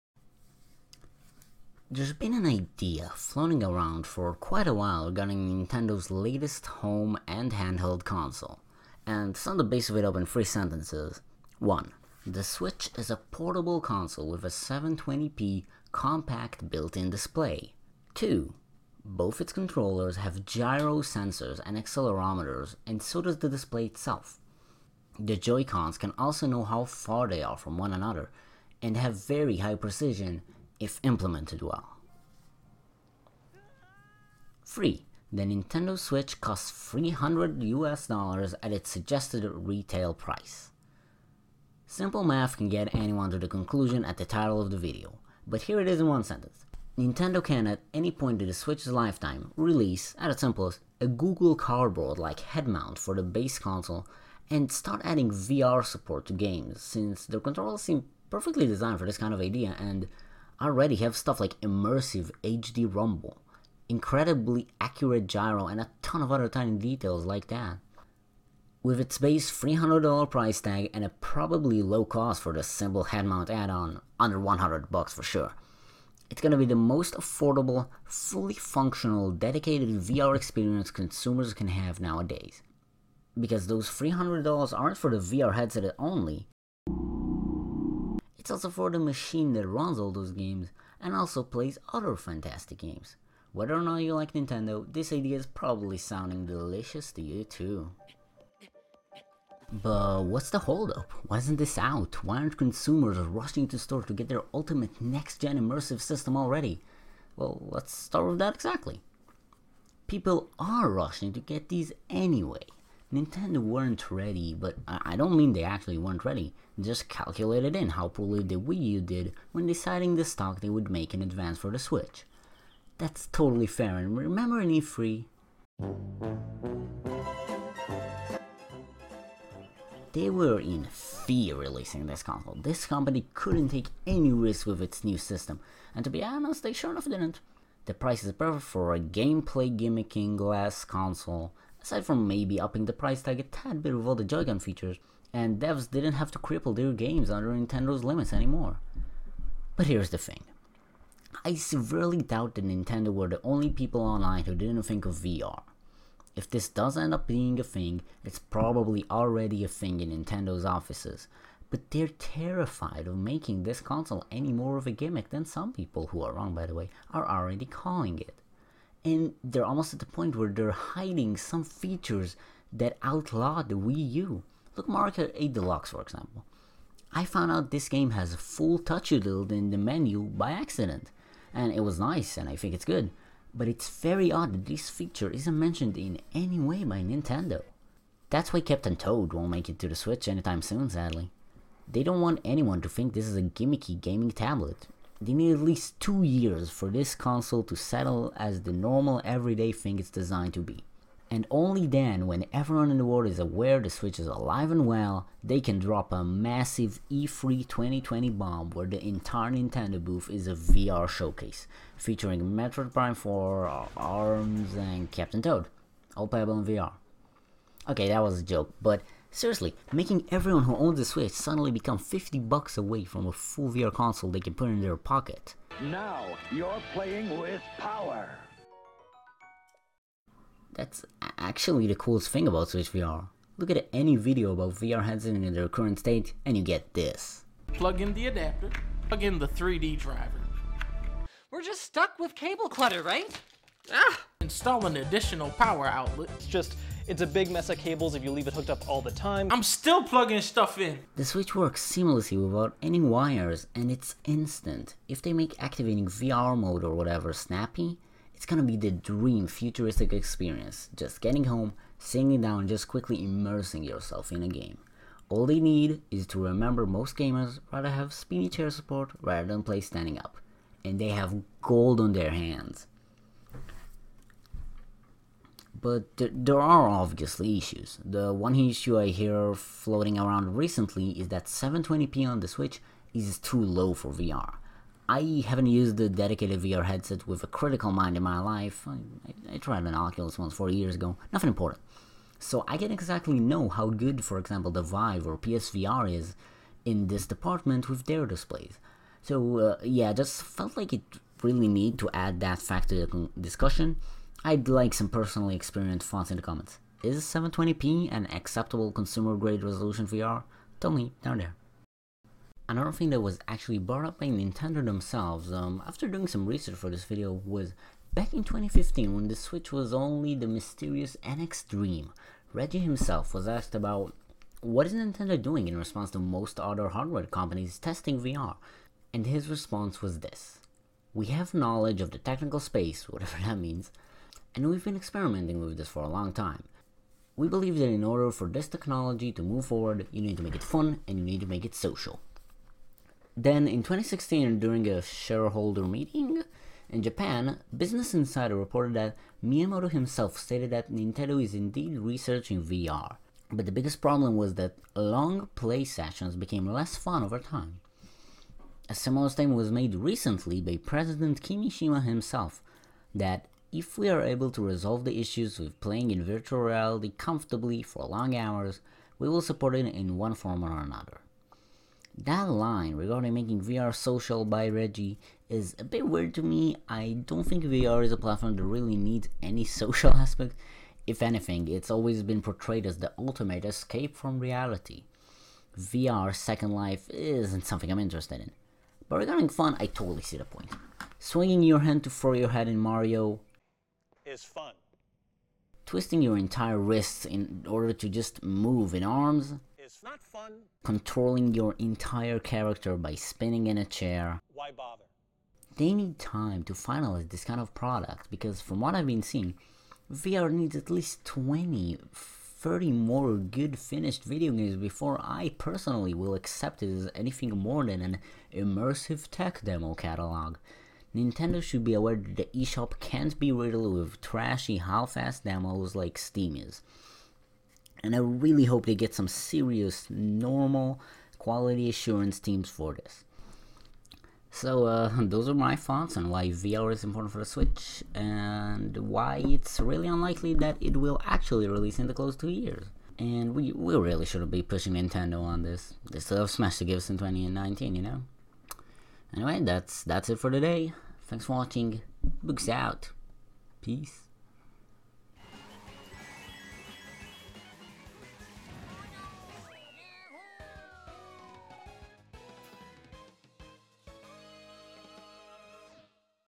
Switch VR - Why it isn't here, And how it needs to arrive
Virtual Reality is an interesting genre of games that raised many eyebrows over the past few years, for good and bad alike.
Is Nintendo gonna dip their toes in this new shiny dimension of gaming, or should they take the slow and steady route and add it once it's an industry standard? Let's have a look.
SOURCES:
(^^ this wasnt related but the clip helped)
and Nintendo's E3 Spotlight which you all saw and know how to find.
They / Them